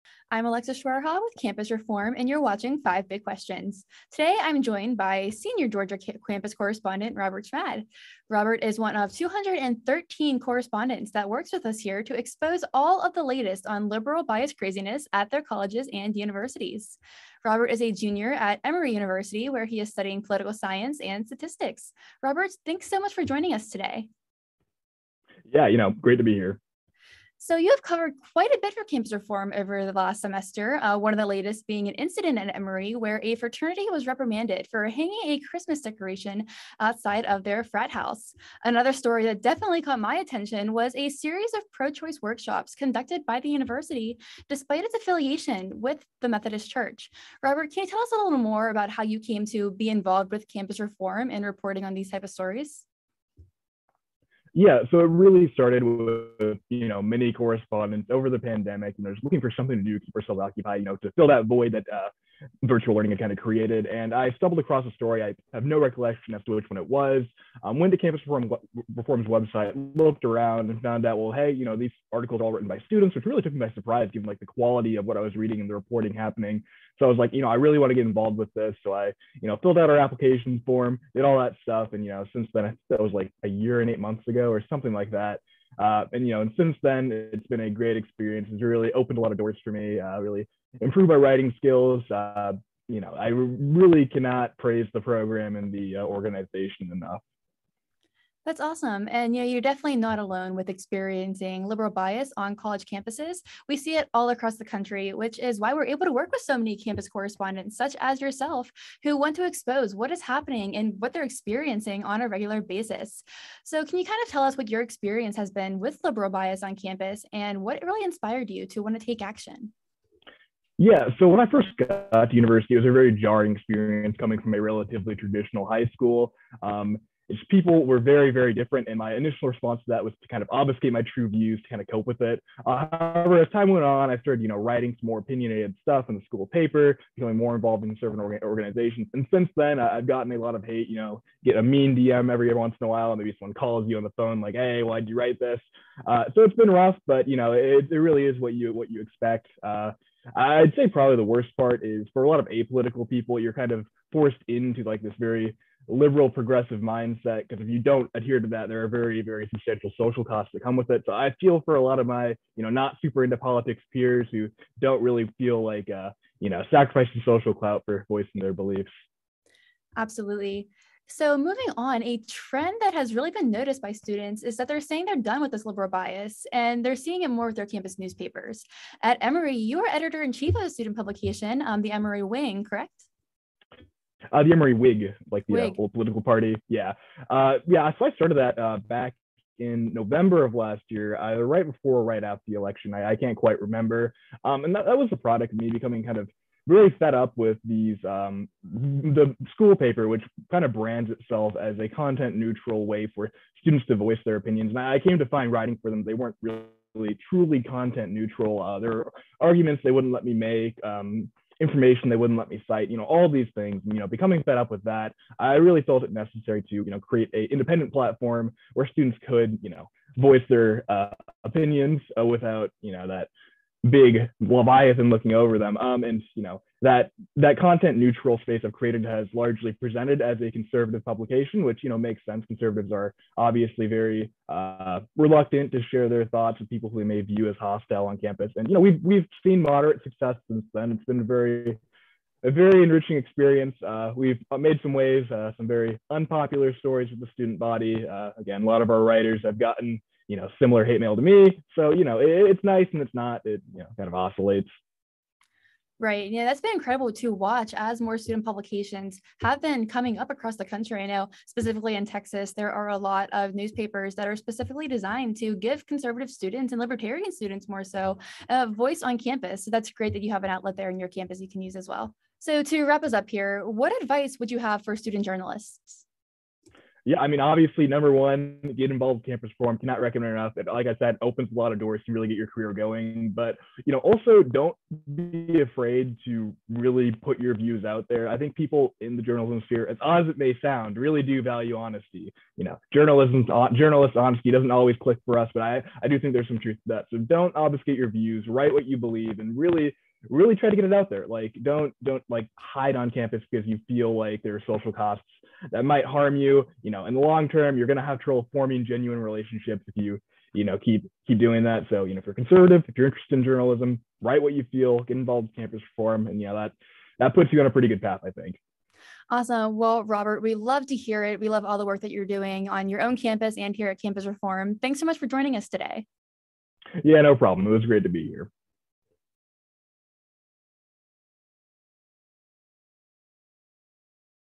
Exposing Liberal Bias Through The Eyes of a Student
Senior Georgia Campus Correspondent Robert Schmad spoke with Campus Reform about his experience in conservative media. Schmad has covered numerous stories for campus reform on and off his campus at Emory University.

In addition to his writing, Schmad also serves as Editor-in-Chief of the Emory Whig, a student-led campus newspaper. With this, he discusses his experience navigating the student publication field, and explained why it is necessary to establish a platform that upholds the value of freedom of press.

"I really felt it necessary to create an independent where students could voice their opinions," Schmad explained.